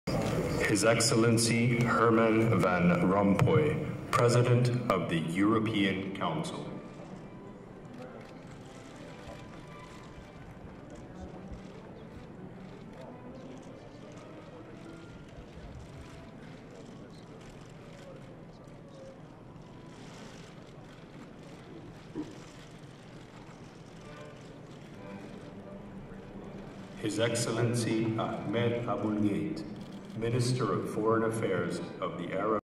At the Nuclear Safety Summit
In representation of the EU the President of the European Council Herman Van Rompuy assists to the Nuclear Safety Summit in Washington which aims to strengthen the fight against nuclear terrorism.